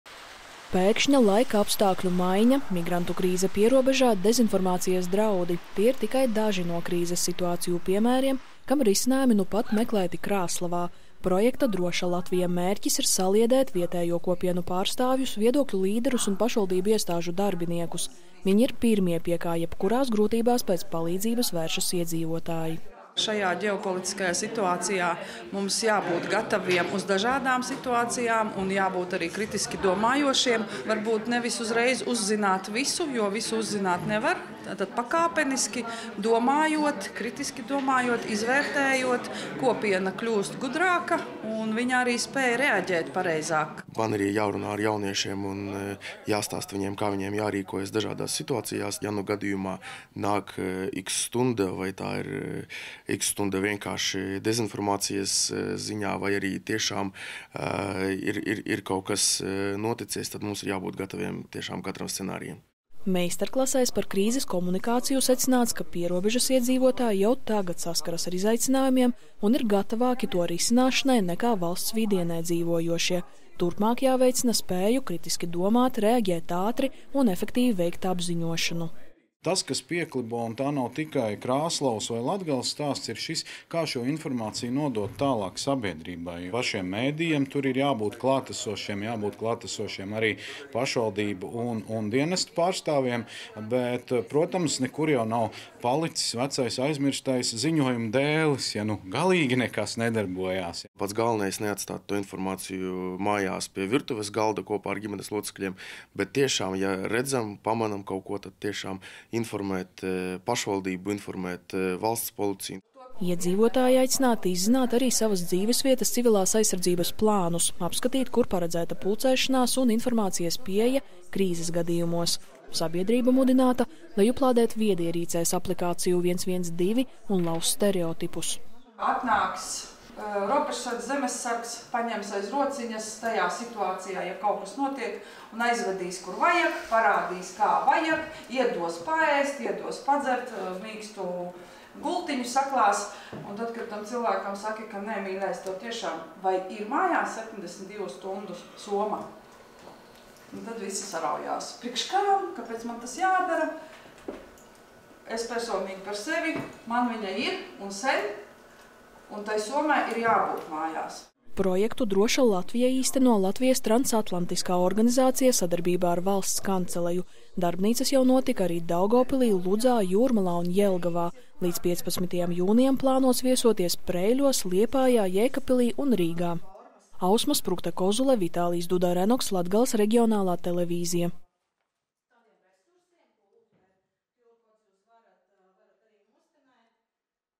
Krāslavā izspēlē un analizē krīzes situācijas
Izspēlējot dažādas krīzes situācijas un kopīgi meklējot tām risinājumus meistarklasēs, deviņas pašvaldības visā Latvijā piedalās informācijas drošības projektā DrošaLatvija. Krāslavā secināts, ka pierobežas aktīvākajiem iedzīvotājiem un nevalstisko organizāciju pārstāvjiem izpratne un gatavība rīcībai krīzes apstākļos ir lielāka, nekā citviet valstī dzīvojošajiem.